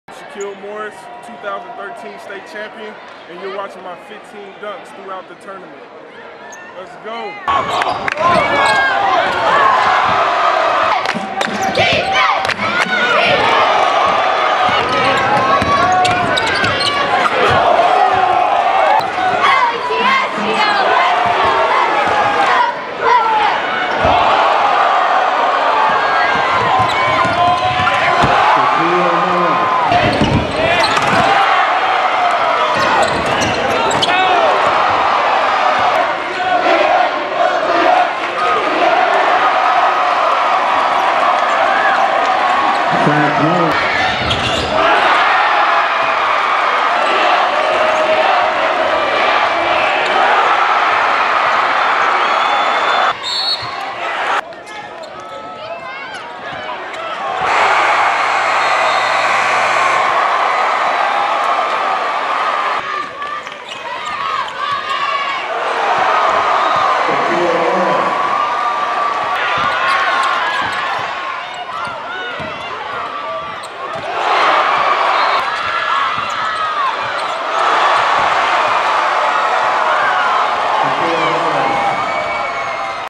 Shaquille Morris' 15 Dunks at the State Tournament
Edmond Memorial big man Shaquille Morris attempts to tear down the Mabee Center's rims, as he leads his Bulldogs to the 2013 Class 6A State Championship.